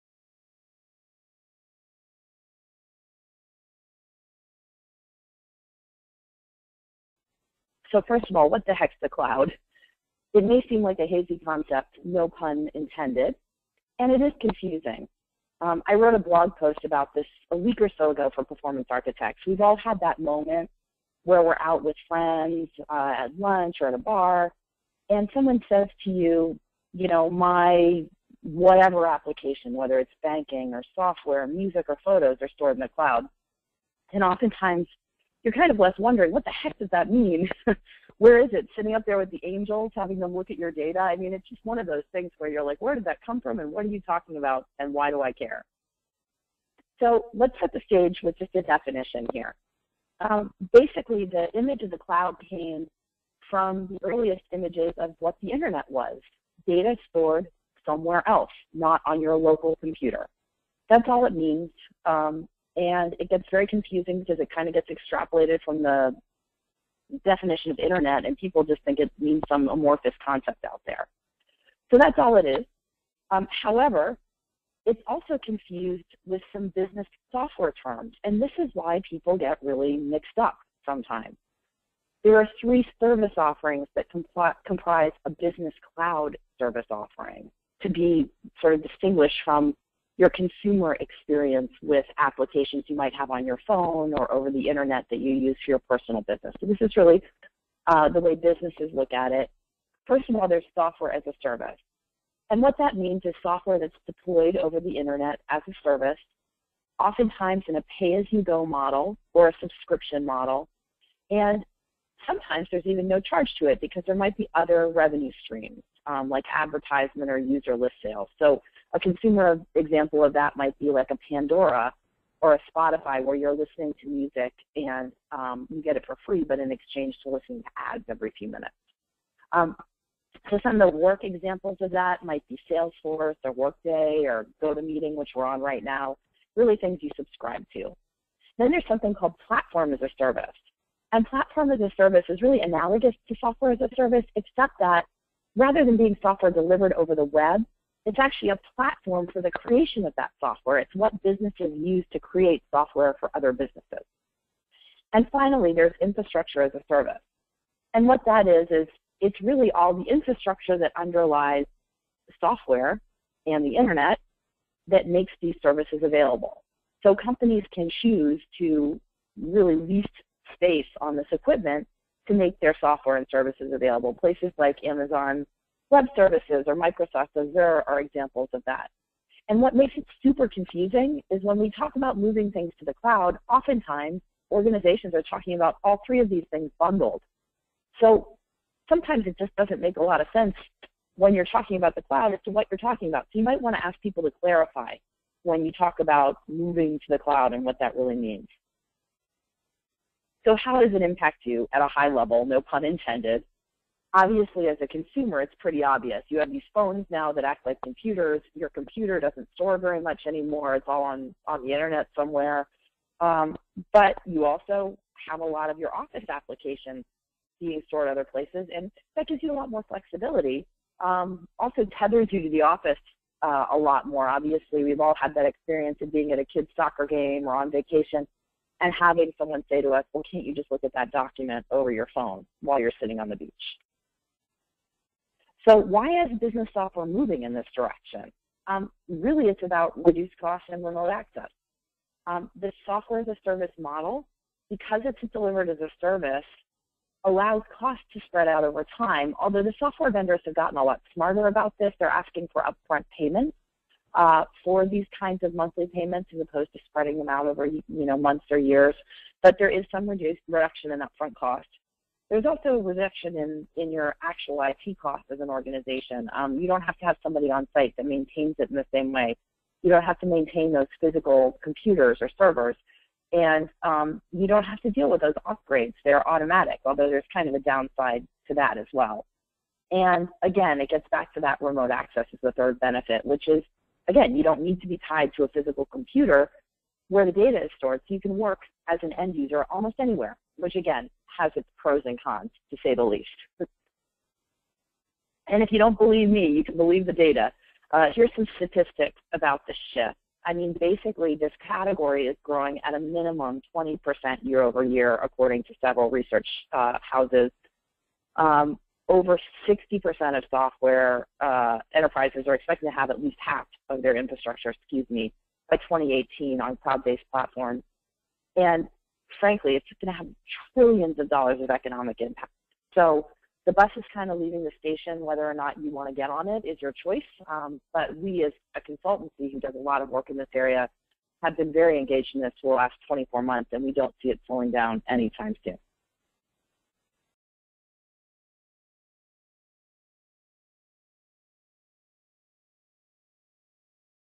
How to Start Planning, Budgeting and Forecasting in the Cloud
Do you plan, budget and forecast in spreadsheets or an old “on-premise” planning software solution today? Tired of trying to get IT support to maintain your planning process? Curious about “cloud” (software-as-a-service or SaaS) planning solutions and how they work? Want a “quick start” process that doesn’t require your entire department’s budget? Then this webinar is for you! Join the Performance Architects team as we discuss real-life case studies on how to get started and what to watch out for when budgeting, forecasting and planning using cloud solutions!